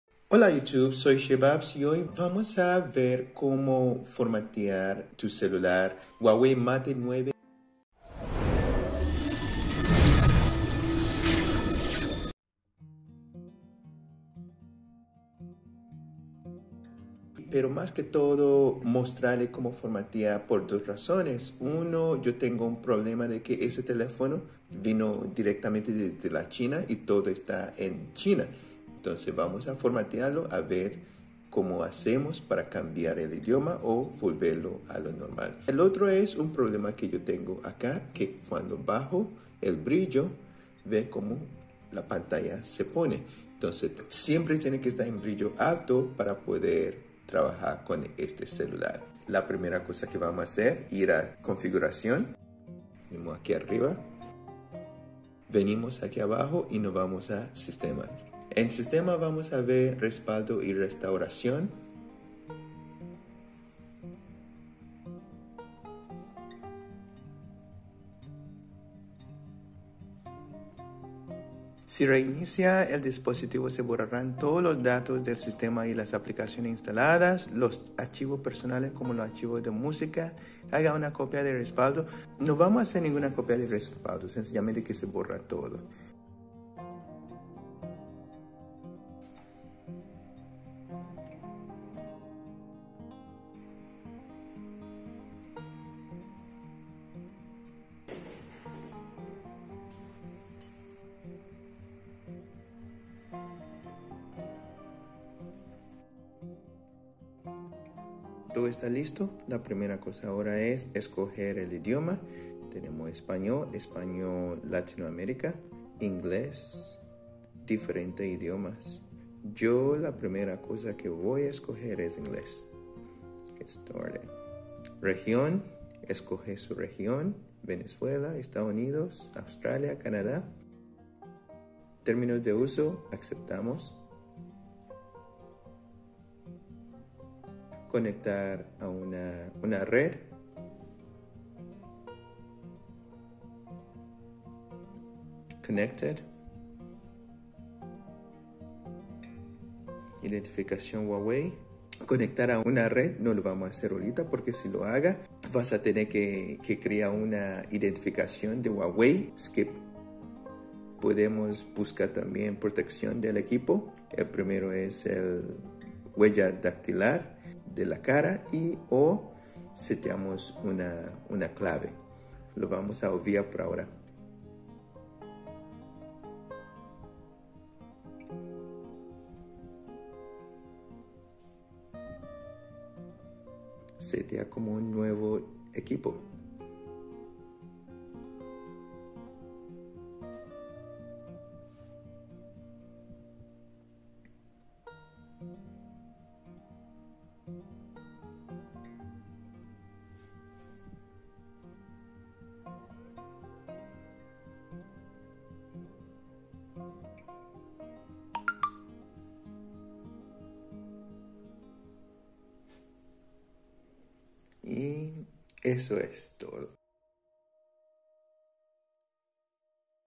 Como formatear tu celular Huawei (Huawei Mate 9)
Este video se hizo para ciertas personas alla afuera que han pedido y que han solicitado la informacion.